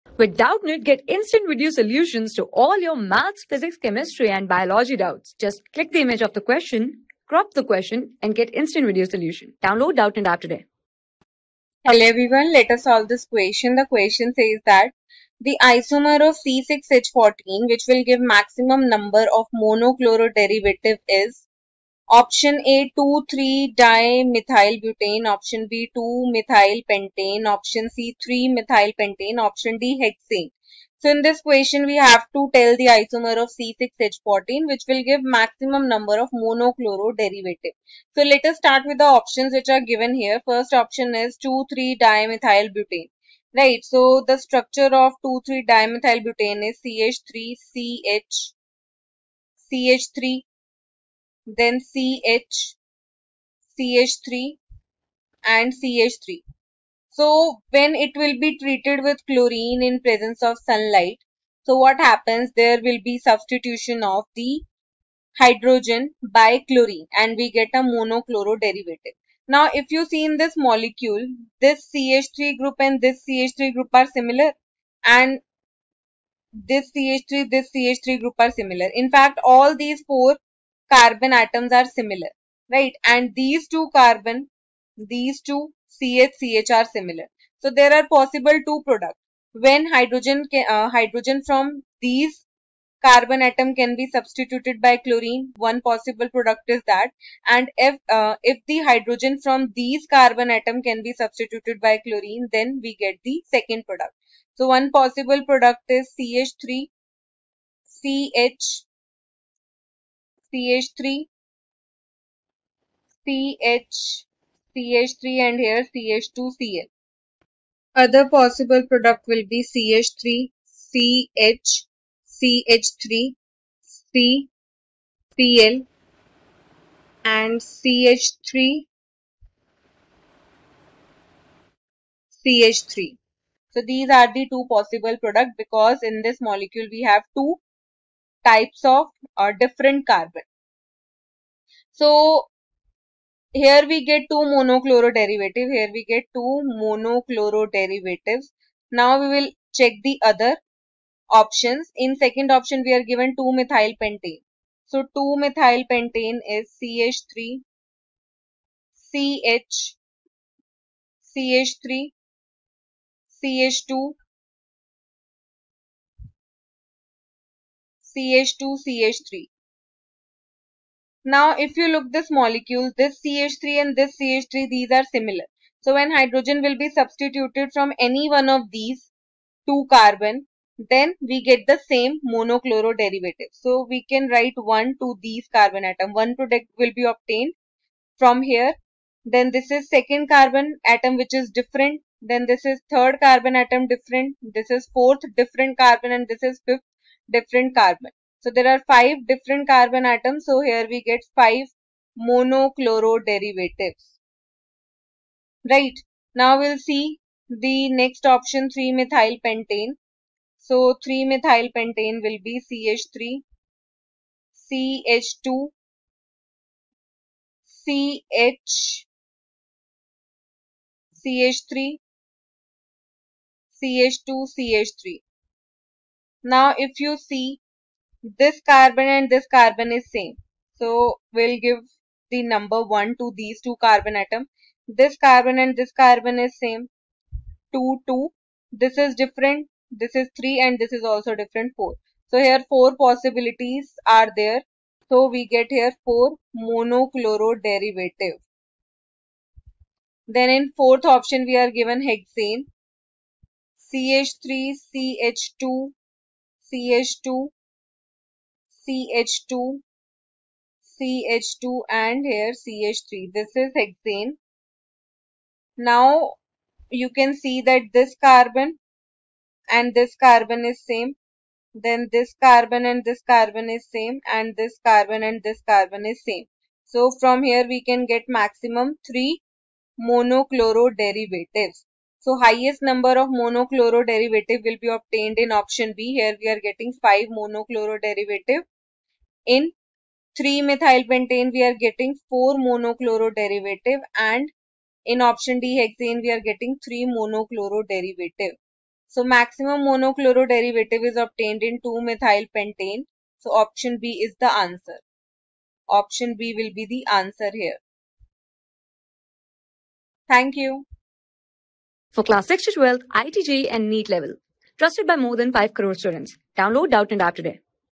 The isomer of C_(6)H_(14) which will give maximum number of monochloroderivative is | 12 | HALO...
The isomer of C_(6)H_(14) which will give maximum number of monochloroderivative is
Class: 12
Subject: CHEMISTRY
Chapter: HALOALKANES AND HALOARENES
Board:IIT JEE

👉 Have Any Query? Ask Us.